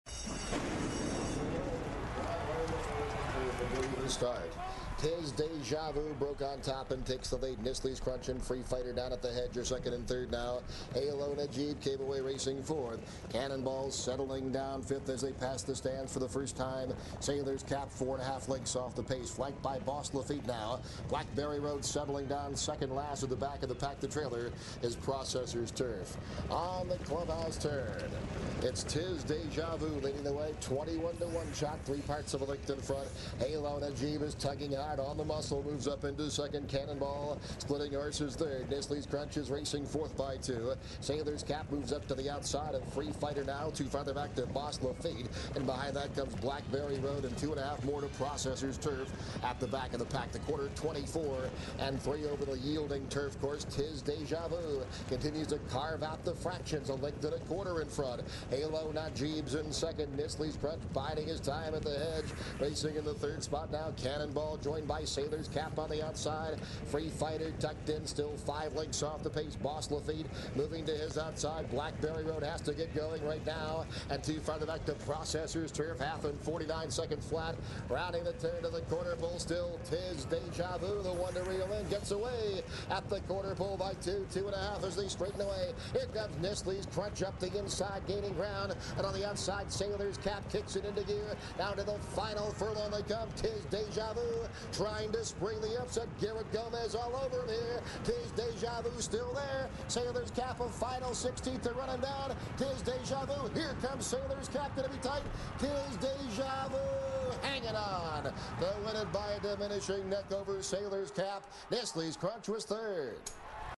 2008 Crown Royal American Turf Stakes
Tizdejavu ($44.60) upsets the Grade 3, $174,450 Crown Royal American Turf Stakes for 3-year-olds at Churchill Downs on May 2, 2008.